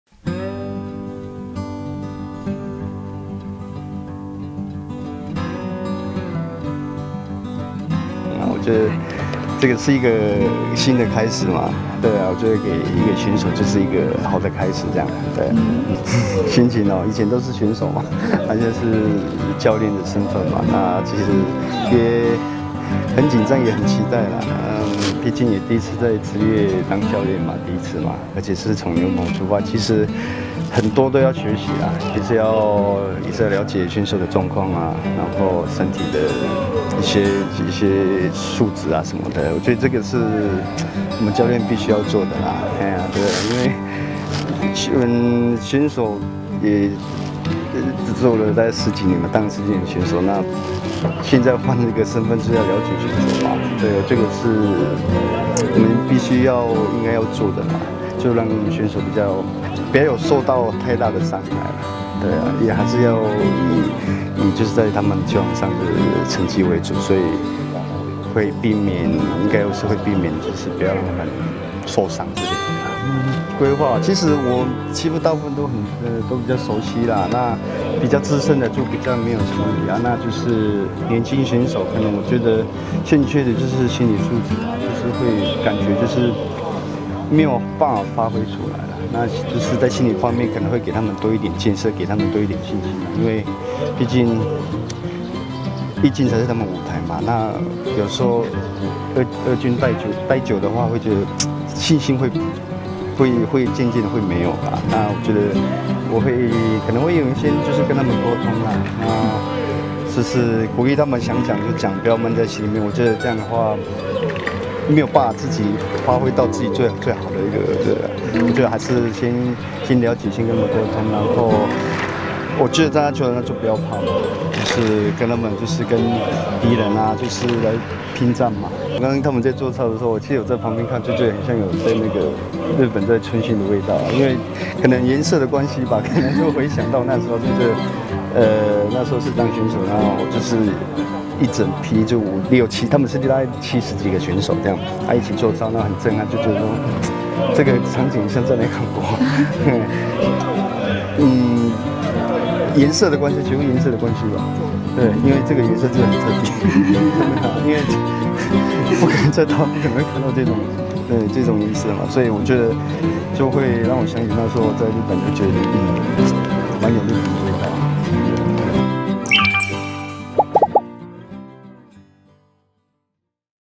【Rakuten開訓】林英傑任牛棚教練 見「樂天紅」很有感觸
Rakuten MONKEYS樂天桃猿展開春訓～
林英傑認為除了球技外，年輕球員最須先建立信心！
並笑稱看到樂天球衣，讓自己忍不住回想起旅日時光～